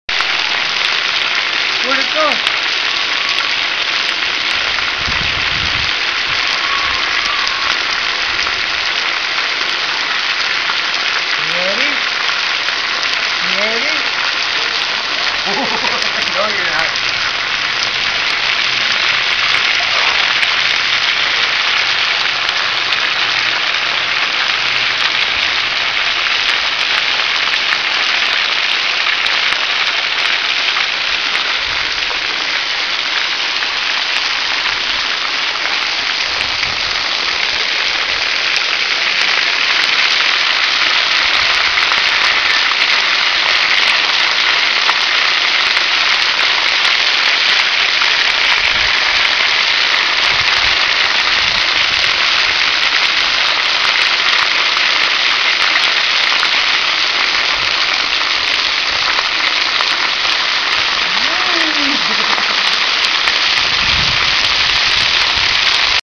Water fun with the baby
Ben playing with baby Benjamin in the water for the first time.